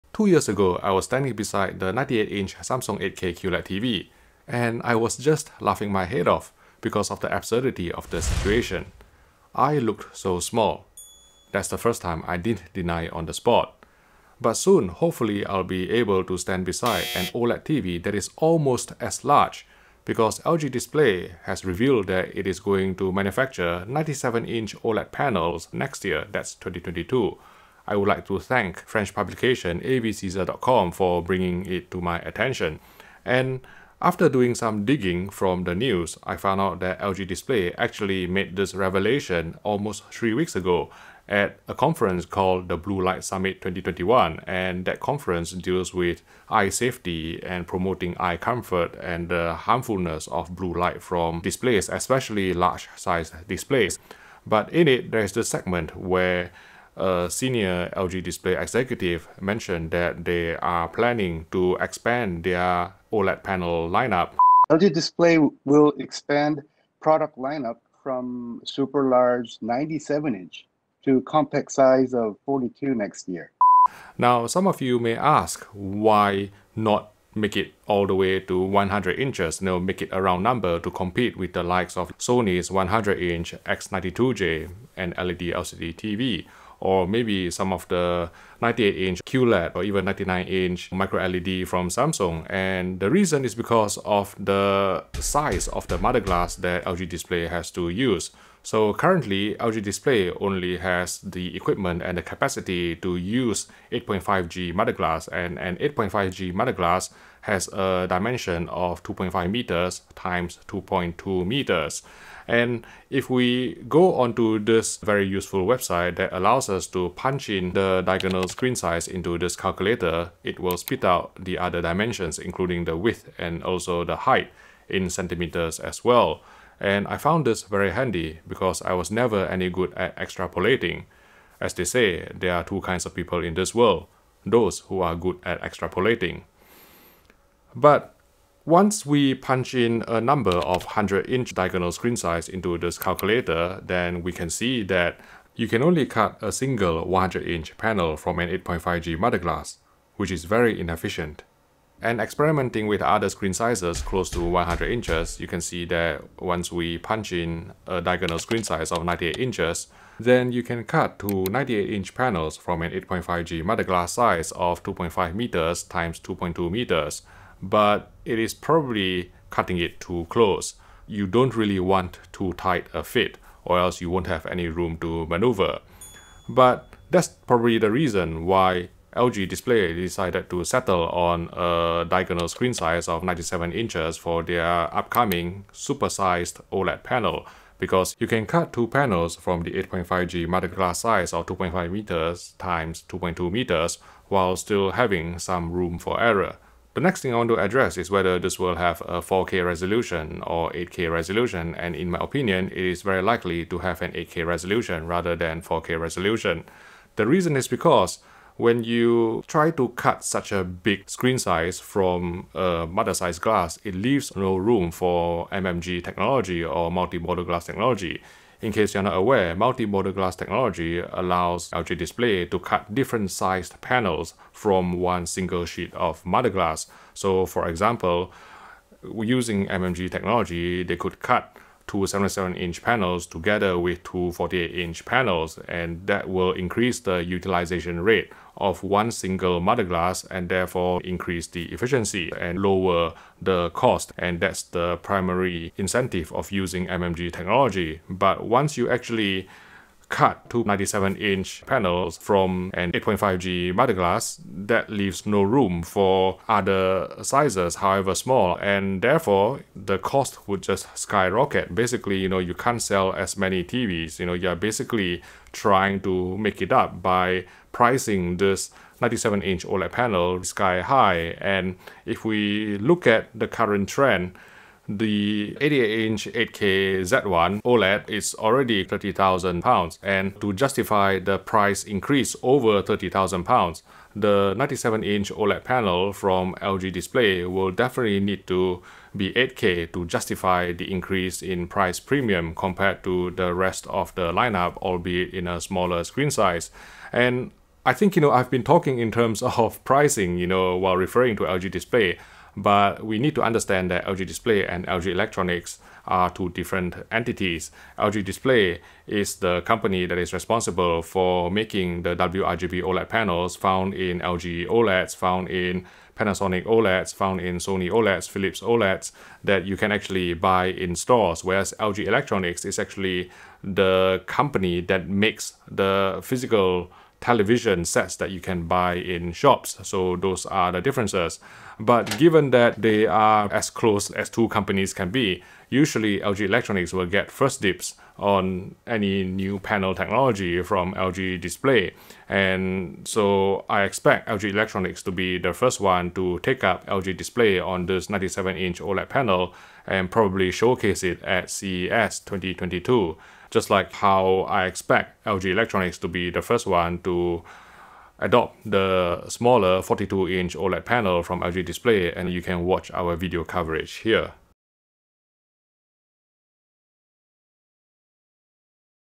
LG to Launch 97-inch OLED TV in 2022, But Why Not 100-inch?
LG Display has revealed that it will introduce a 97-inch OLED panel next year, and so we expect LG Electronics to launch a 97in OLED TV in 2022. Although the screen resolution is not specified, it's likely to be 8K rather than 4K to justify the significant price premium.

We also explain why LG stopped short of 100 inches, and had to settle with *only* a 97-inch OLED panel.